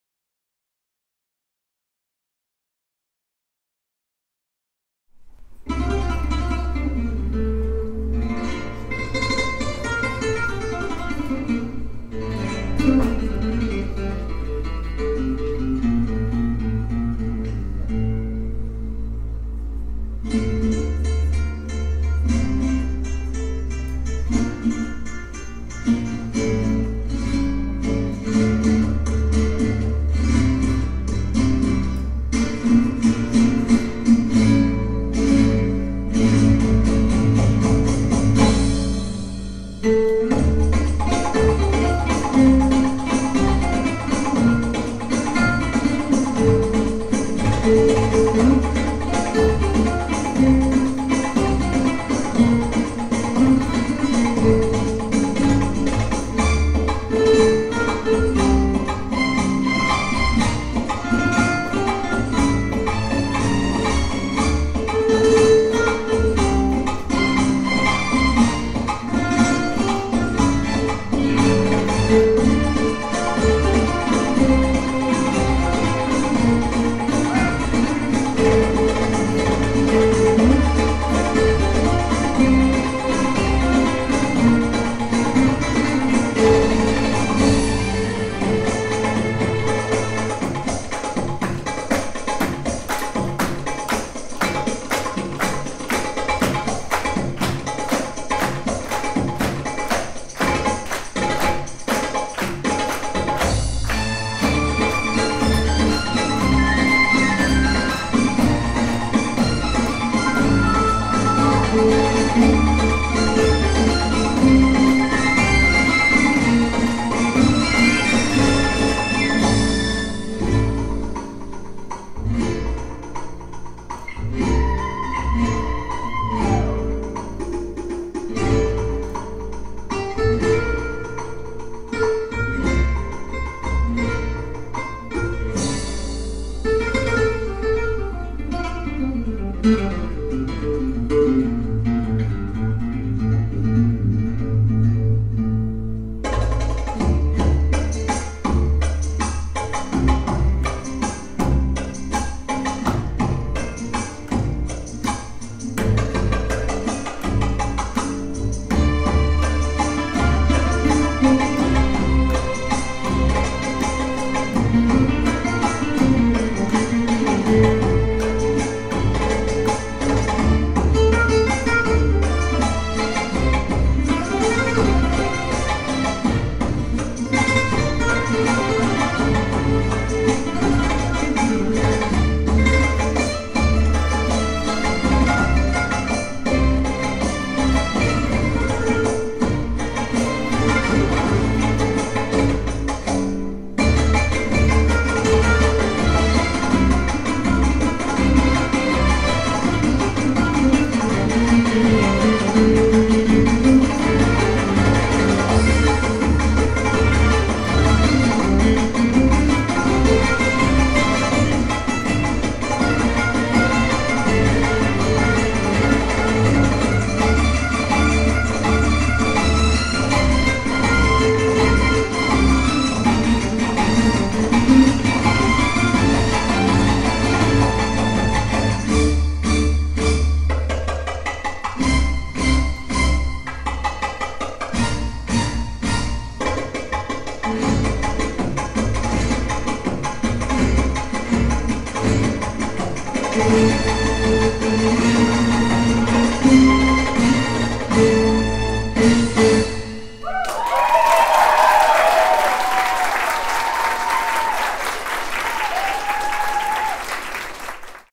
Bellydonna Spanish fusion
Belladonna performing at the Banat el Kamar annual spring concert. May 30, 2015